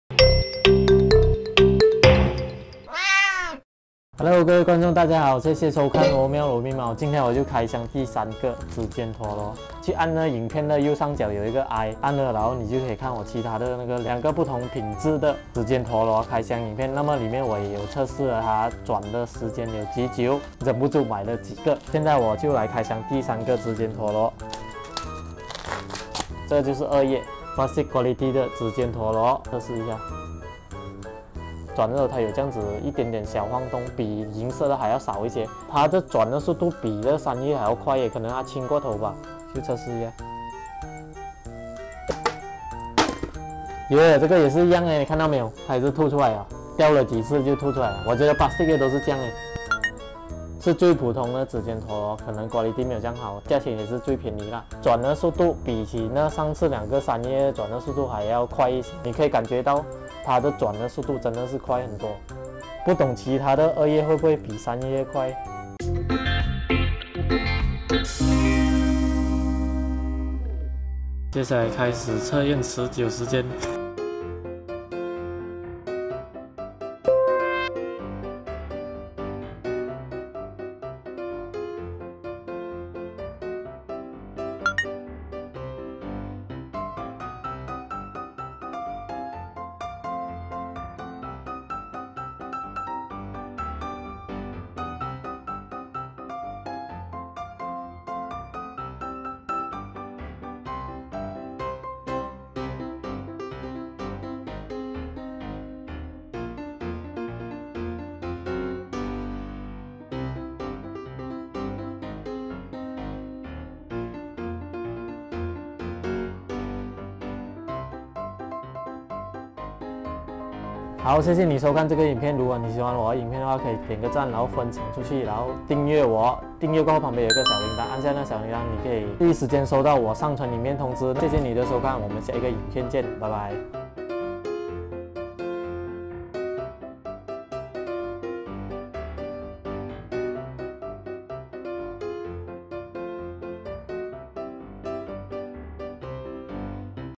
【開箱】|二葉指尖陀螺！感覺轉起來比較快！|RoMeow 儸密貓
【影片介紹】
大家好！這是我另一個指尖陀螺開箱，已經幾個月了還是一直看到指尖陀螺的影片一直出現。近期看到的是外國指尖陀螺的變身戰鬥影片，有點像是懞面超人變身然後對打的畫面，老實說效果還做得蠻好的。上網找了之間陀螺才發現有很多款式很有趣！都想把好看的都收集了起來。哈哈！於是就買了幾個來試試玩看看，這是類似鋁品質的指尖陀螺，就試看看轉起來會是什麽樣的感覺。那麽大家就來看看這個指尖陀螺的開箱影片吧。
希望大家會喜歡這個影片。 :D

【RoMeow 儸密貓鏈接】
Youtube 頻道 / Youtube Channel